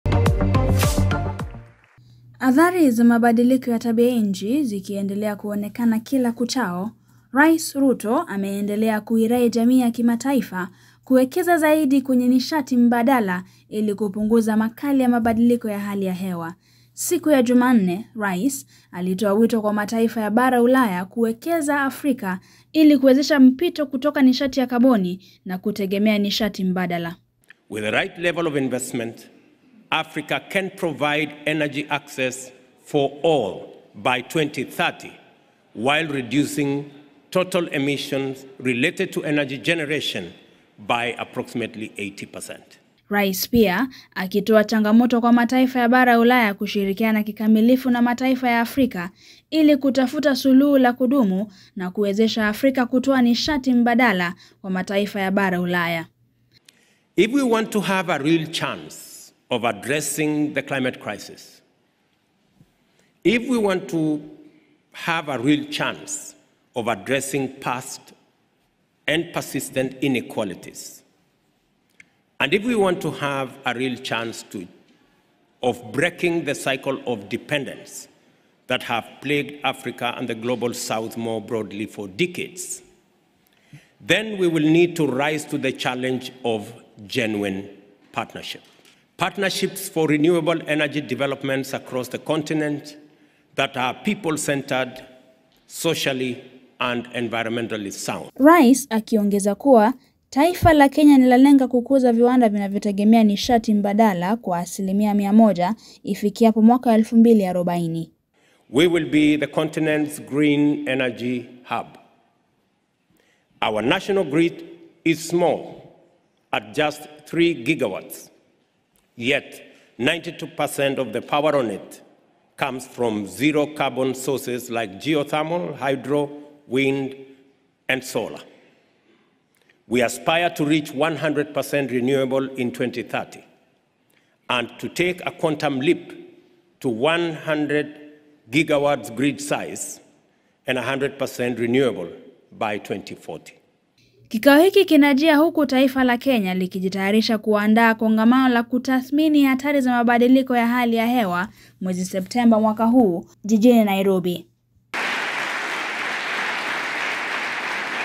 KUKABILI MABADILIKO TABIANCHI
Rais atoa wito kwa mataifa  ya Bara Ulaya kushirikiana na Afrika kukuza nishati mbadala.